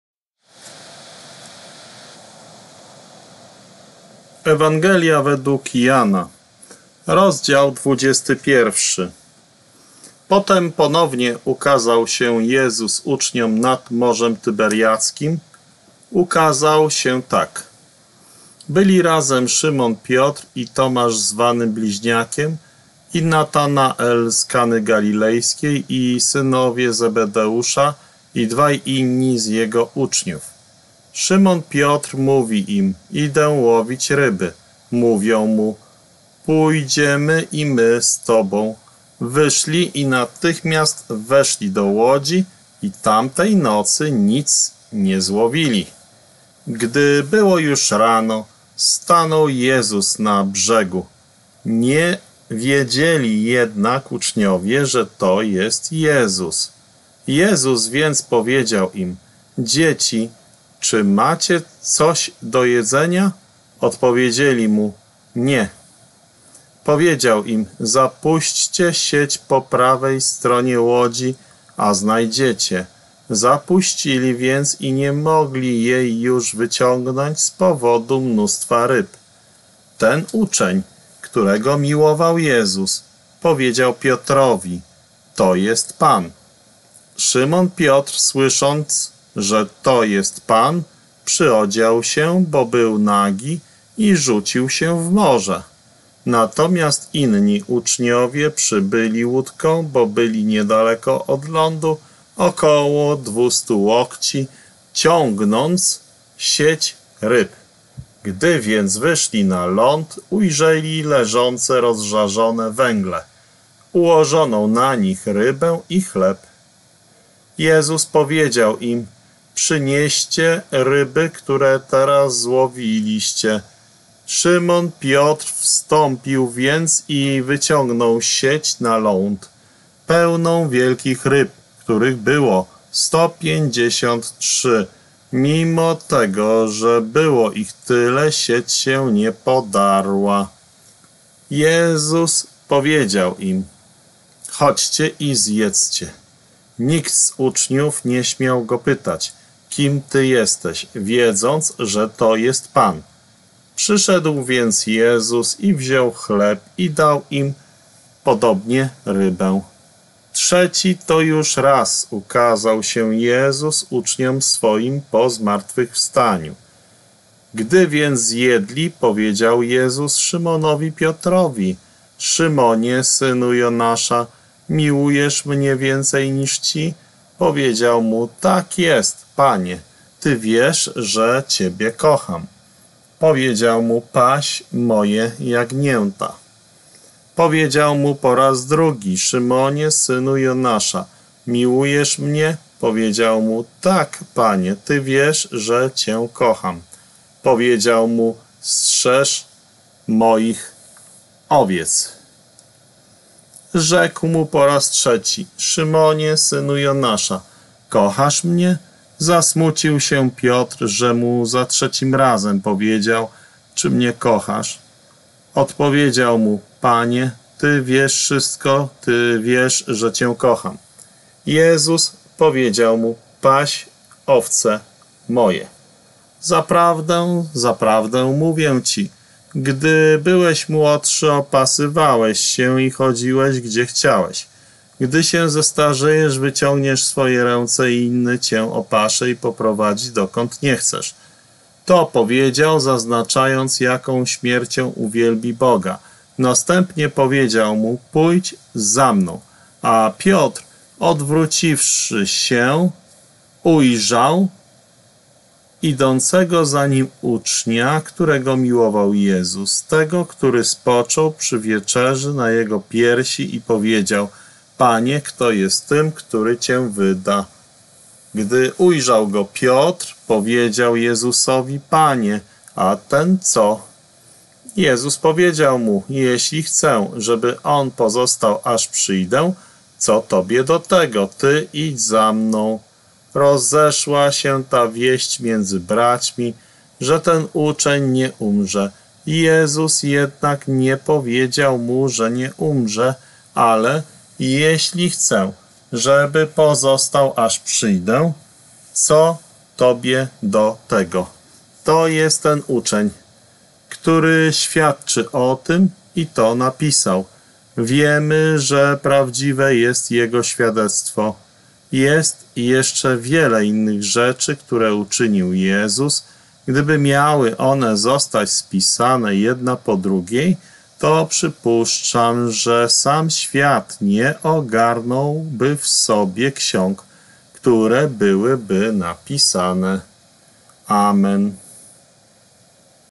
BBO Biblia Brzesko Ostrzeszowska przekład z Textus Receptus BBO43 Ewangelia wg Jana rozdział 21
papierowa wersja:

#1 Potem ponownie ukazał się Jezus uczniom nad morzem Tyberiadzkim. Ukazał się tak:
#2 Byli razem Szymon Piotr i Tomasz zwany Bliźniakiem, i Natanael z Kany Galilejskiej, i synowie Zebedeusza, i dwaj inni z jego uczniów.
#3 Szymon Piotr mówi im: Idę łowić ryby. Mówią mu: Pójdziemy i my z tobą. Wyszli i natychmiast weszli do łodzi i tamtej nocy nic nie złowili.
#4 Gdy było już rano, stanął Jezus na brzegu. Nie wiedzieli jednak uczniowie, że to jest Jezus.
#5 Jezus więc powiedział im: Dzieci! Czy macie coś do jedzenia? Odpowiedzieli mu: Nie.
#6 Powiedział im: Zapuśćcie sieć po prawej stronie łodzi, a znajdziecie. Zapuścili więc i nie mogli jej już wyciągnąć z powodu mnóstwa ryb.
#7 Ten uczeń, którego miłował Jezus, powiedział Piotrowi: To jest Pan. Szymon Piotr, słysząc, że to jest Pan, przyodział się, bo był nagi i rzucił się w morze.
#8 Natomiast inni uczniowie przybyli łódką, bo byli niedaleko od lądu, około dwustu łokci, ciągnąc sieć ryb.
#9 Gdy więc wyszli na ląd, ujrzeli leżące rozżarzone węgle, ułożoną na nich rybę i chleb.
#10 Jezus powiedział im: Przynieście ryby, które teraz złowiliście.
#11 Szymon Piotr wstąpił więc i wyciągnął sieć na ląd, pełną wielkich ryb, których było sto pięćdziesiąt trzy. Mimo tego, że było ich tyle, sieć się nie podarła.
#12 Jezus powiedział im: Chodźcie i zjedzcie. Nikt z uczniów nie śmiał go pytać: Kim ty jesteś? Wiedząc, że to jest Pan.
#13 Przyszedł więc Jezus i wziął chleb, i dał im, podobnie rybę.
#14 Trzeci to już raz ukazał się Jezus uczniom swoim po zmartwychwstaniu.
#15 Gdy więc zjedli, powiedział Jezus Szymonowi Piotrowi: Szymonie, synu Jonasza! Miłujesz mnie więcej niż ci? Powiedział mu: Tak jest, Panie! Ty wiesz, że ciebie kocham. Powiedział mu: Paś moje jagnięta.
#16 Powiedział mu po raz drugi: Szymonie, synu Jonasza! Miłujesz mnie? Powiedział mu: Tak, Panie! Ty wiesz, że cię kocham. Powiedział mu: Strzeż moich owiec.
#17 Rzekł mu po raz trzeci: Szymonie, synu Jonasza! Kochasz mnie? Zasmucił się Piotr, że mu za trzecim razem powiedział: Czy mnie kochasz? Odpowiedział mu: Panie! Ty wiesz wszystko, ty wiesz, że cię kocham. Jezus powiedział mu: Paś owce moje.
#18 Zaprawdę, zaprawdę mówię ci: Gdy byłeś młodszy, opasywałeś się i chodziłeś, gdzie chciałeś. Gdy się zestarzejesz, wyciągniesz swoje ręce i inny cię opasze i poprowadzi, dokąd nie chcesz.
#19 To powiedział, zaznaczając, jaką śmiercią uwielbi Boga. Następnie powiedział mu: Pójdź za mną.
#20 A Piotr, odwróciwszy się, ujrzał idącego za nim ucznia, którego miłował Jezus, tego, który spoczął przy wieczerzy na jego piersi i powiedział: Panie, kto jest tym, który cię wyda?
#21 Gdy ujrzał go Piotr, powiedział Jezusowi: Panie! A ten co?
#22 Jezus powiedział mu: Jeśli chcę, żeby on pozostał, aż przyjdę, co tobie do tego? Ty idź za mną!
#23 Rozeszła się ta wieść między braćmi, że ten uczeń nie umrze. Jezus jednak nie powiedział mu, że nie umrze, ale: Jeśli chcę, żeby pozostał, aż przyjdę, co tobie do tego?
#24 To jest ten uczeń, który świadczy o tym i to napisał. Wiemy, że prawdziwe jest jego świadectwo.
#25 Jest jeszcze wiele innych rzeczy, które uczynił Jezus. Gdyby miały one zostać spisane jedna po drugiej, to przypuszczam, że sam świat nie ogarnąłby w sobie ksiąg, które byłyby napisane. Amen.